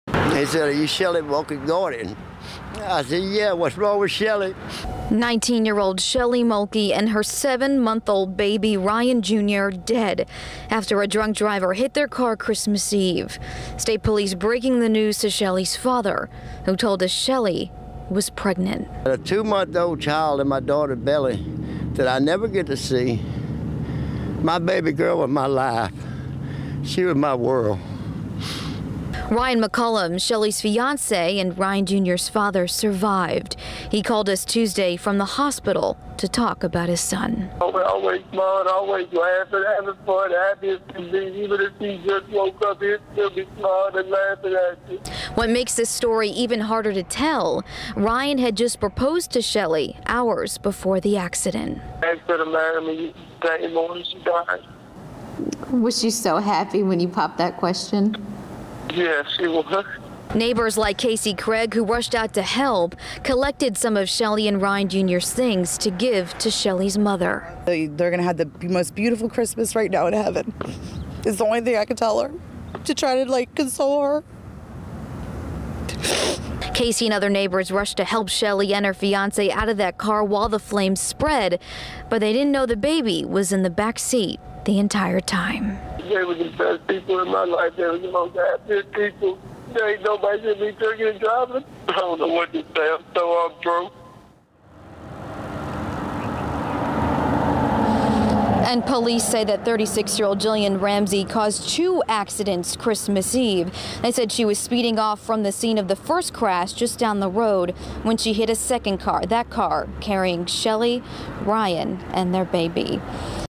Pregnant teen and 7 month old baby killed by drunk driver
A 19-year-old mother and her 7-month old son were killed in a fiery crash on LA 1065 near the intersection of LA 1064.